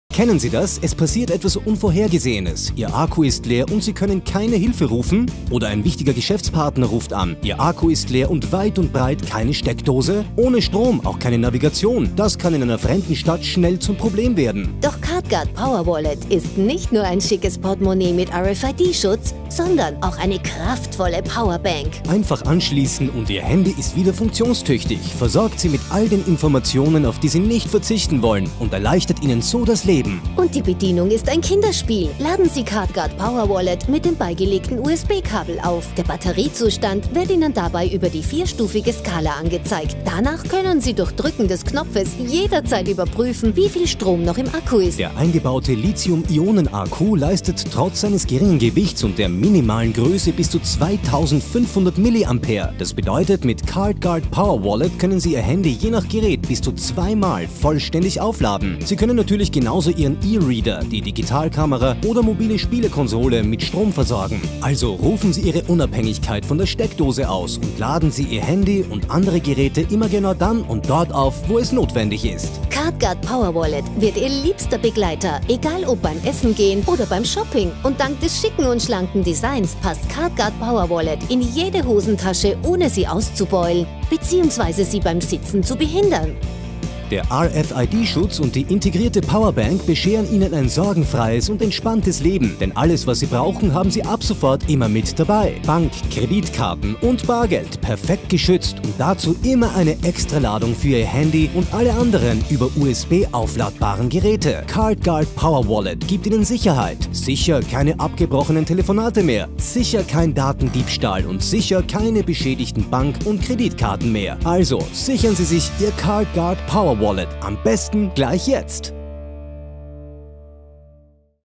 Card Guard Power Wallet - Sicherheits-Portmonnaie mit Ladefunktion
Kraftvoller Akku und schützende Börse!

Mit diesem Geldbeutel von CARD GUARD sind Sie vor Datenklau geschützt und haben, dank integriertem Hochleistungs-Akku, immer eine Ladestation für Ihr Handy dabei! Extrem leichtes Gehäuse aus hochwertigem Aluminium – ideal auf Reisen. Inkl. Mikro-USB Ladekabel.